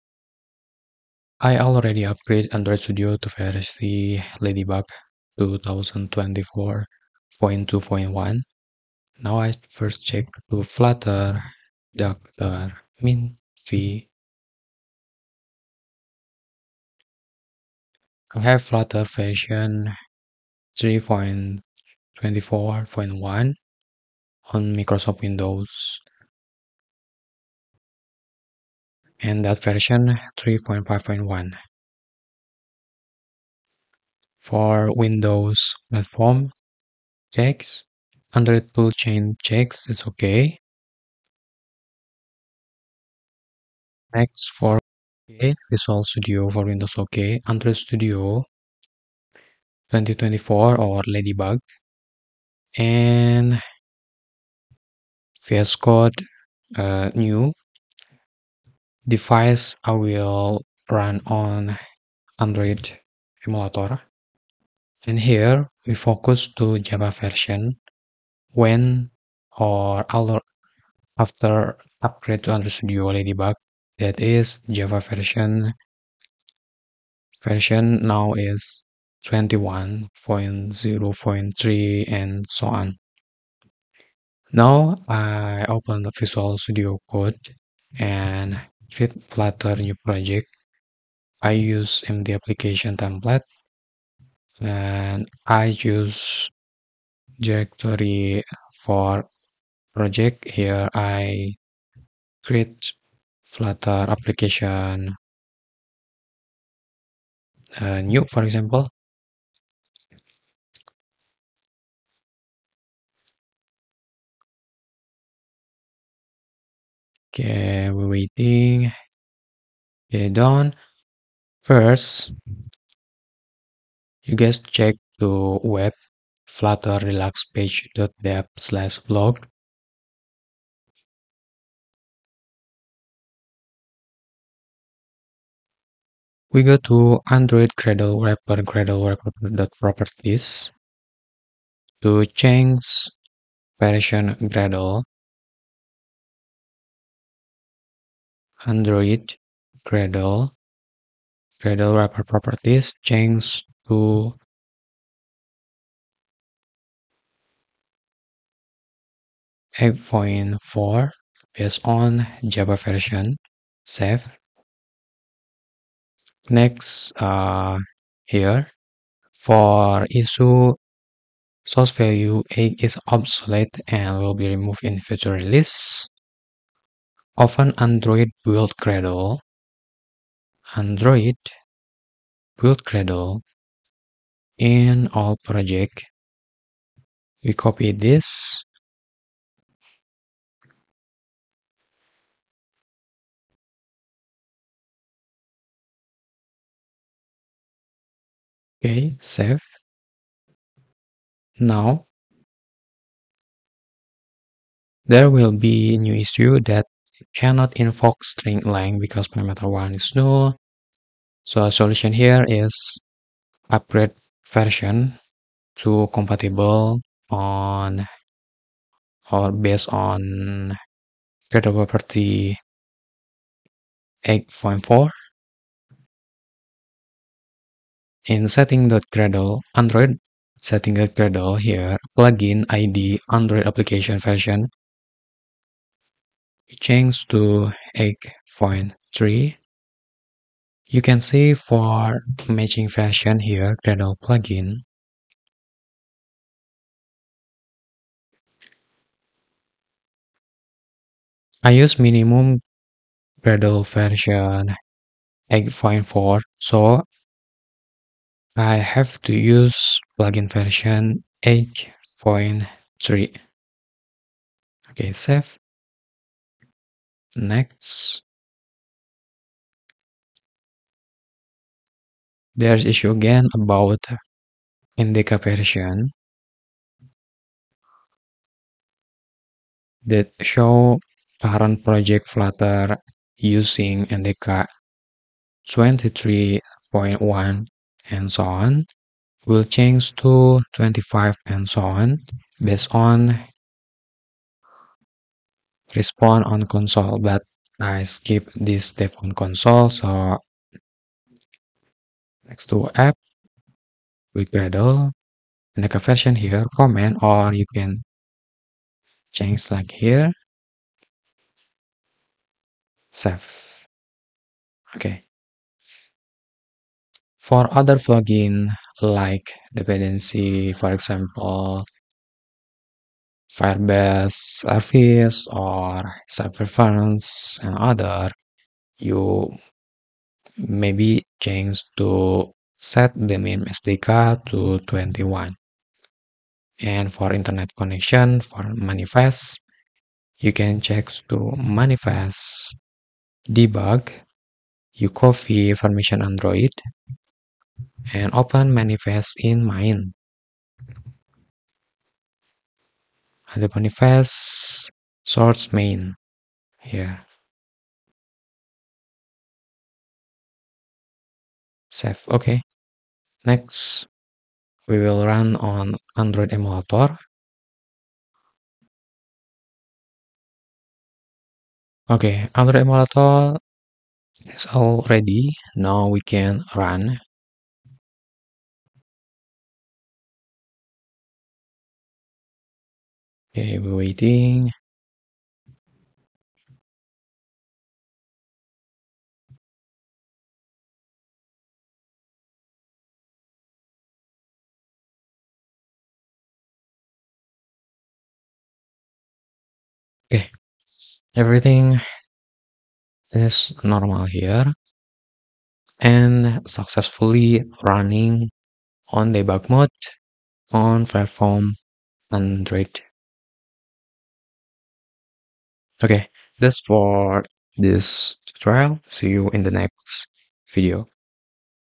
Create Flutter Project After Android Studio Ladybug
After upgrade Android Studio to Ladybug, there are some incompatible version setting on Flutter project, and here how to set compatible version.

Current Spec:
- Android Studio LadyBug
- Java Version 21 (Based on Android Studio)
- Flutter 3.24.1